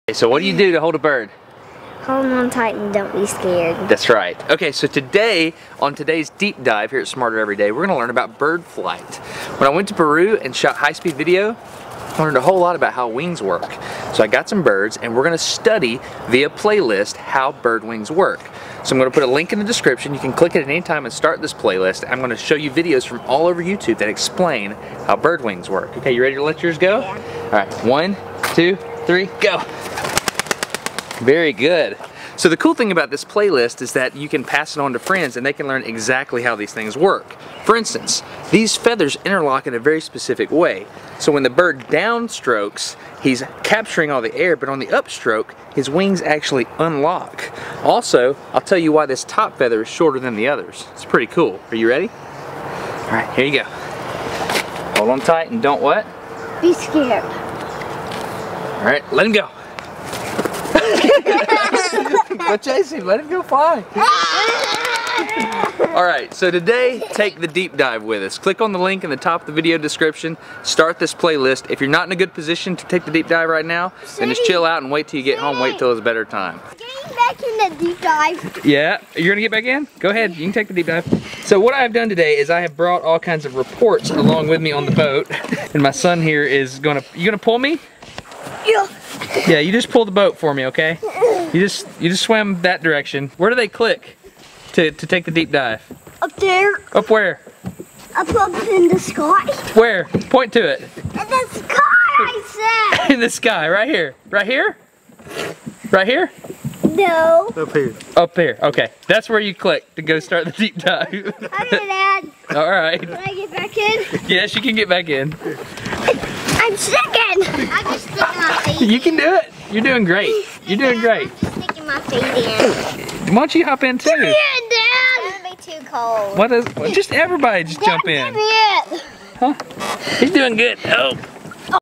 Bird Flight - Deep Dive #2 - Smarter Every Day 61
Click to Start DEEP DIVE!
The Total playlist is only about 25 minutes.

SMARTER EVERY DAY - DEEP DIVE #2

How do you clip a wing?

how to clip your birds wings (with call ducks)
Why clip feathers? An Interview of Kelly

Changing feathers while still being able to fly?

Don't bail out! Keep going!
Biomimicking a Bird?

Huge variety of feather shapes all from the same bird

You Completed Deep Dive #2 !!

❓Mystery Item (just for fun)
Things I use and like: